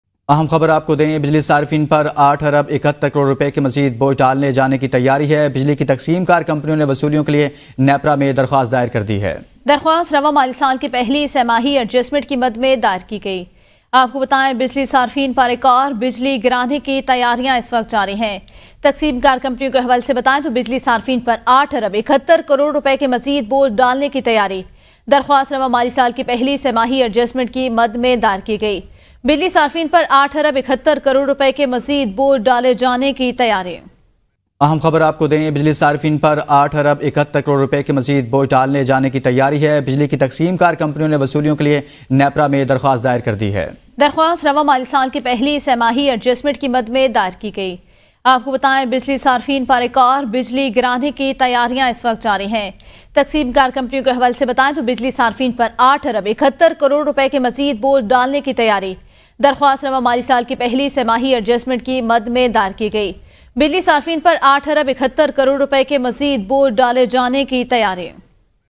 BAD NEWS for Electricity Consumers | Electricity Price Hike | BREAKING NEWS
مشکلات میں گھری عوام کو ایک اور بڑا دھچکا
بجلی صارفین کیلئے بڑا عذاب
قیمتوں میں ایک بار پھر اضافہ